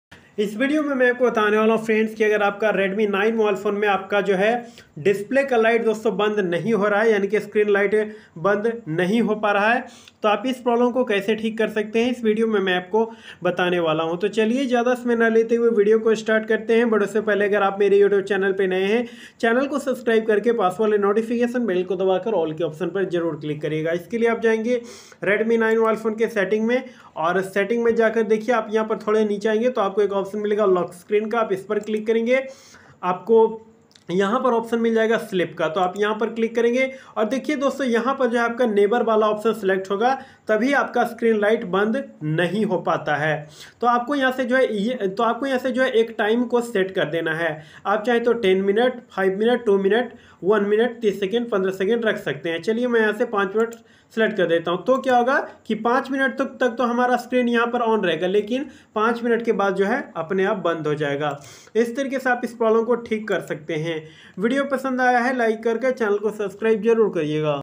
redmi 9 me screen display off nahi ho raha kaise thik kare ।। redmi 9 screen not off problem
is video me bataya gaya hai friends ki redmi 9 mobile me screen off nahi hone ke problem ko kaise thik Kiya jata hai